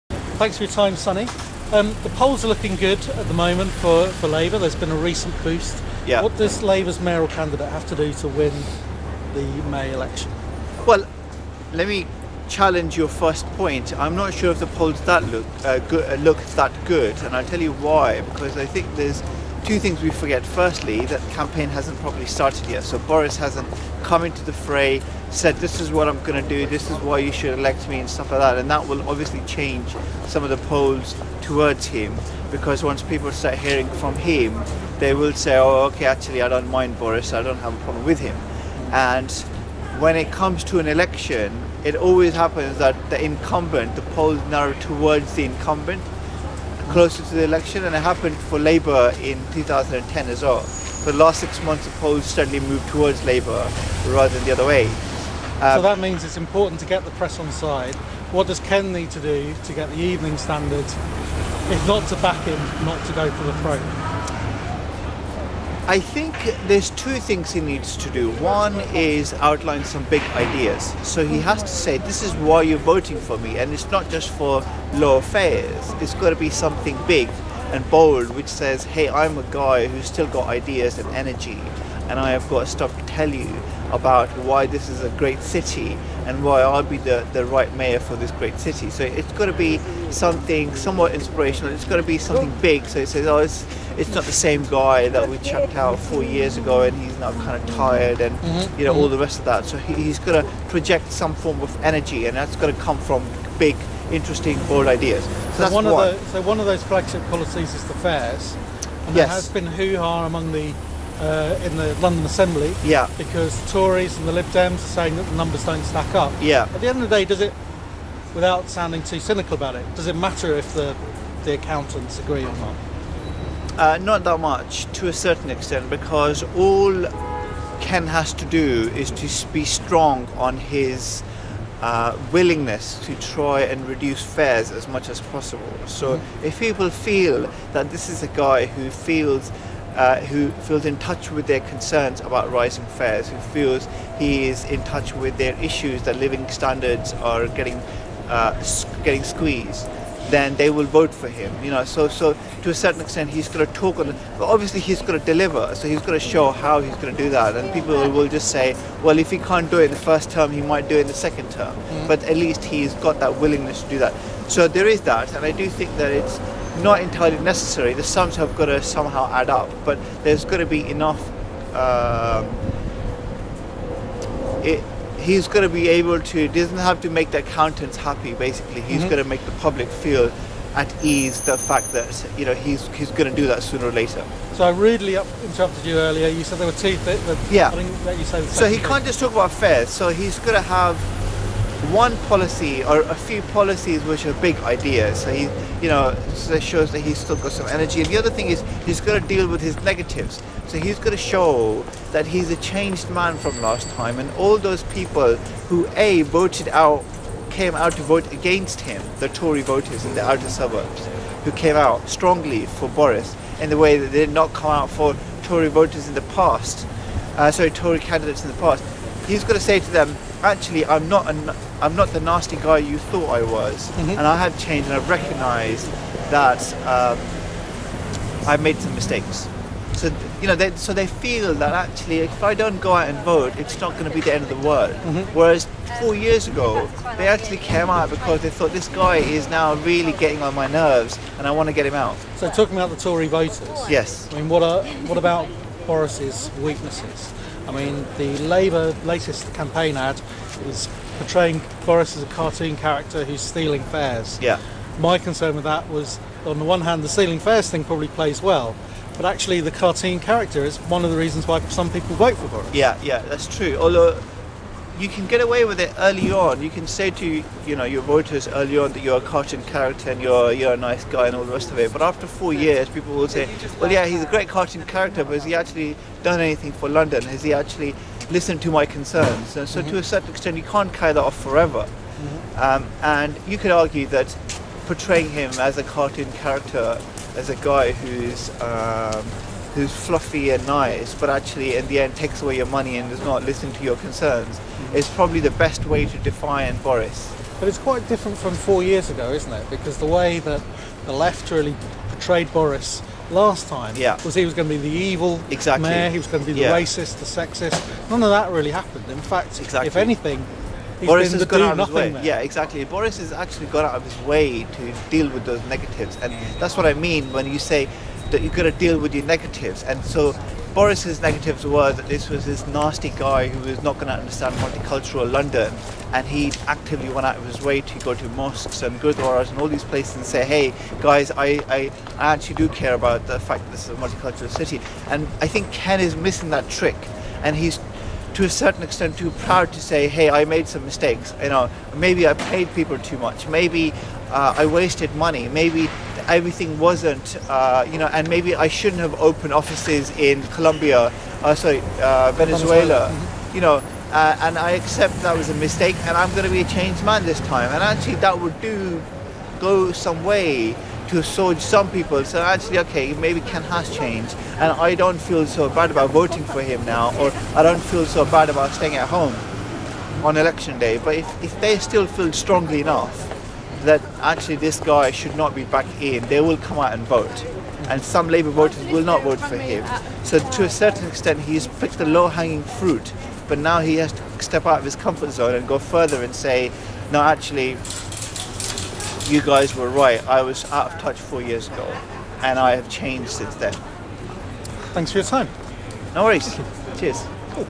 Big Smoke interview with Sunny Hundal
What does Ken Livingstone need to do to get elected? Big Smoke interview with Sunny Hundal, editor of Liberal Conspiracy and contributor to the Guardian newspaper

Feb 2012